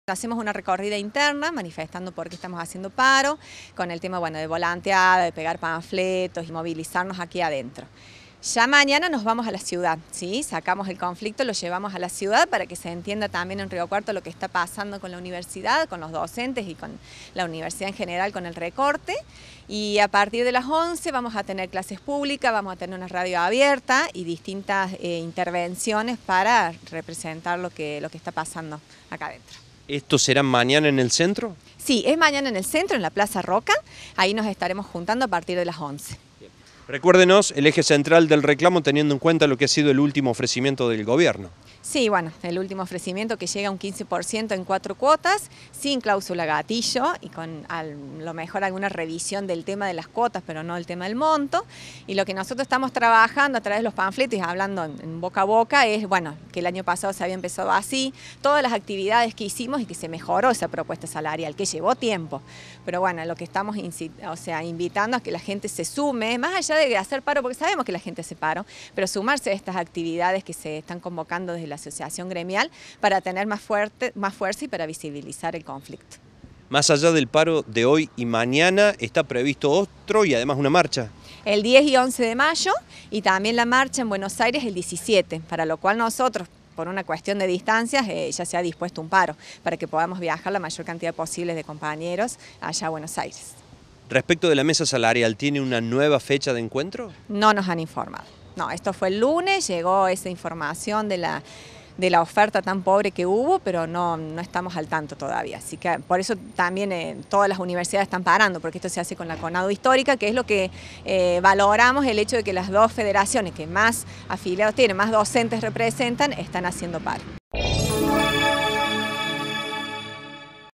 PRIMER DÍA DE PARO DOCENTE - ADRIANA MOYETTA
EL ACATAMIENTO ALCANZA EL 86%
Facebook.com/uniriotv
Twitter.com/uniriotv